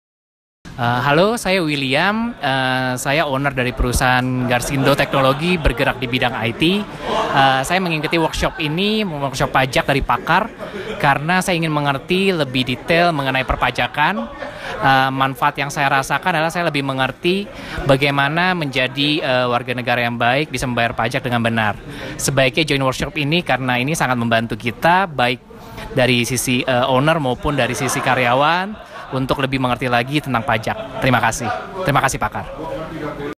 Testimonial Workshop Pajak Pakar (William)
Testimonial William seorang owner dari Garsindo Teknologi setelah mengikuti workshop pajak bersama Pakar.

Testimonial Workshop Pajak PAKAR